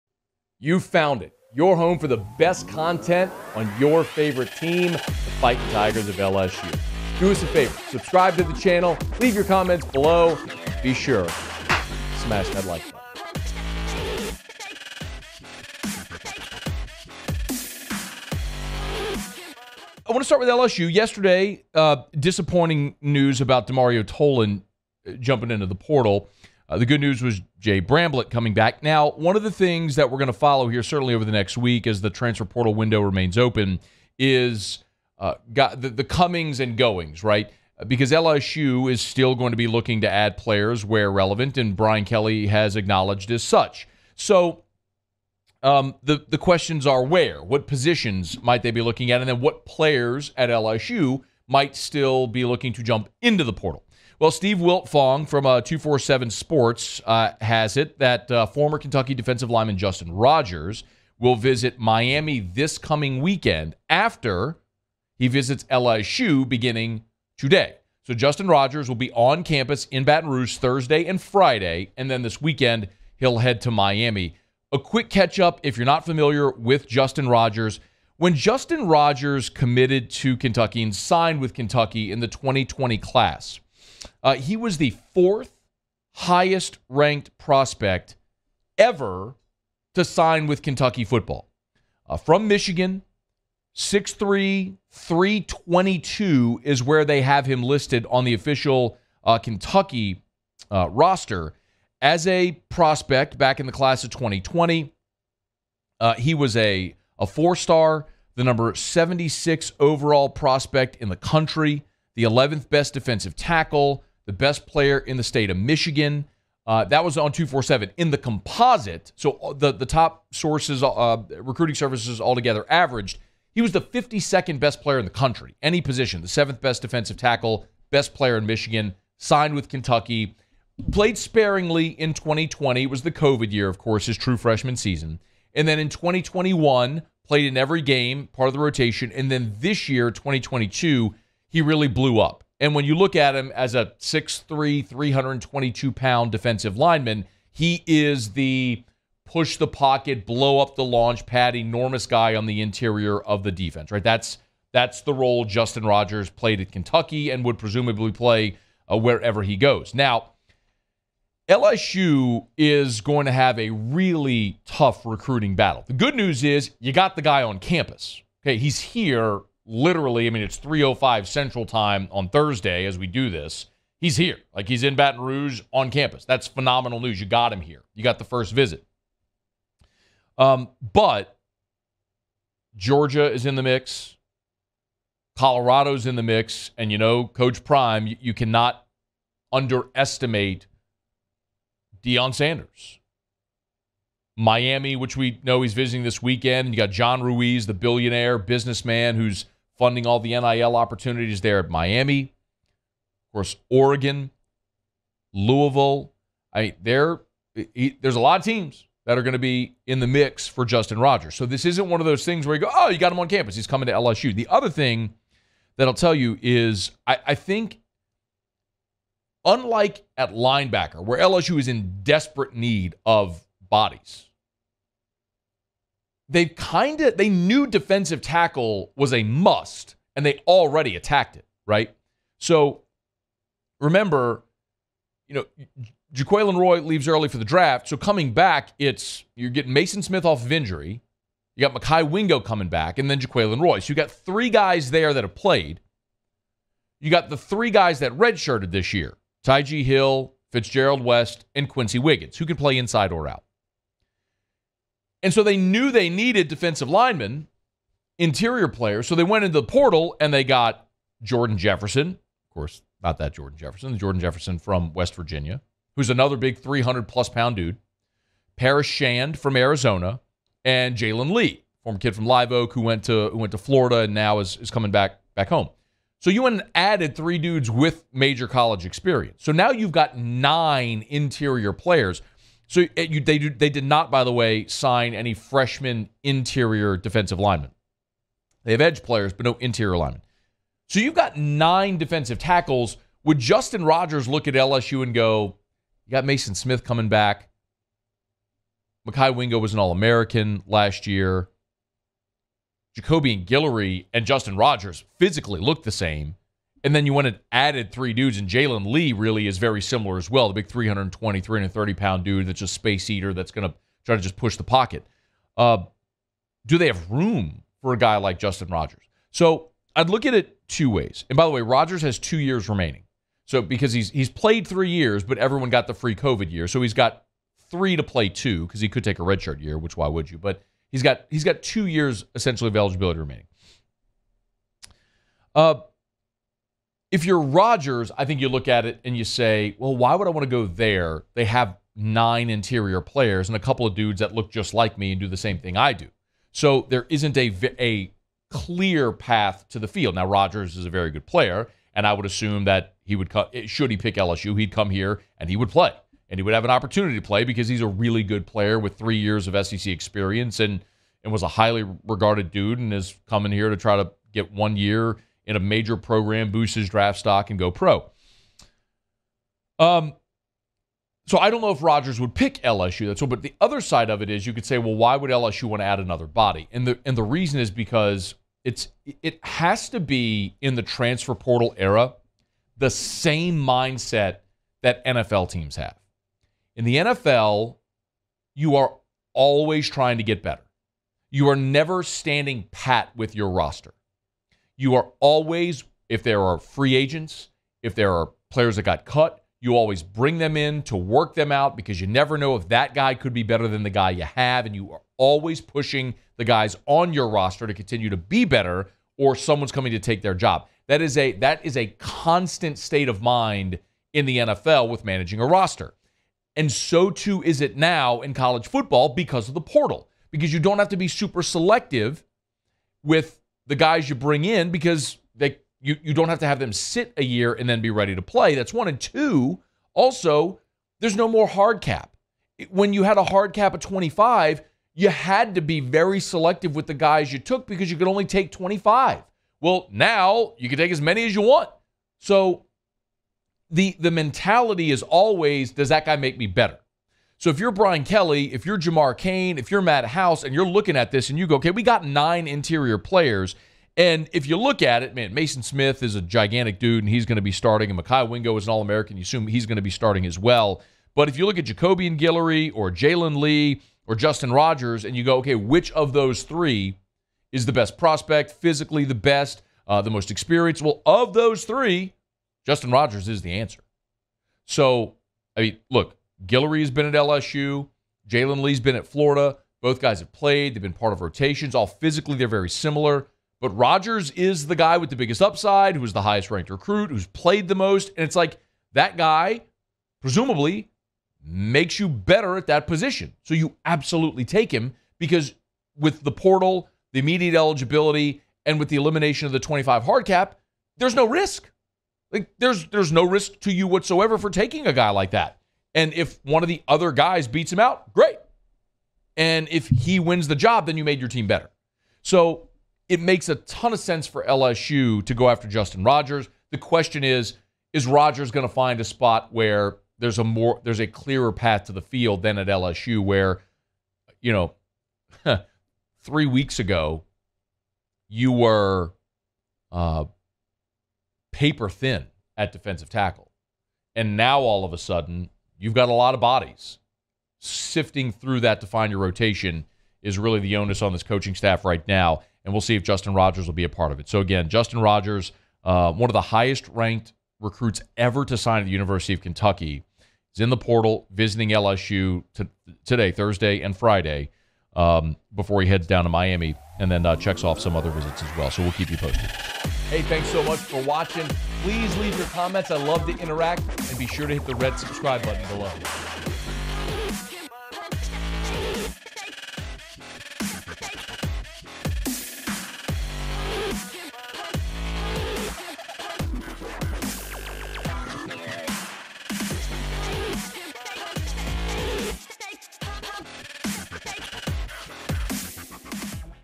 Can LSU Beat UGA for MASSIVE DT Transfer?
Former Kentucky DT Justin Rogers will visit LSU this weekend as a transfer portal prospect. Can LSU get a commit from the former 4-star DT? If so, what does he add to the defensive line depth and how would he fit into the rotation?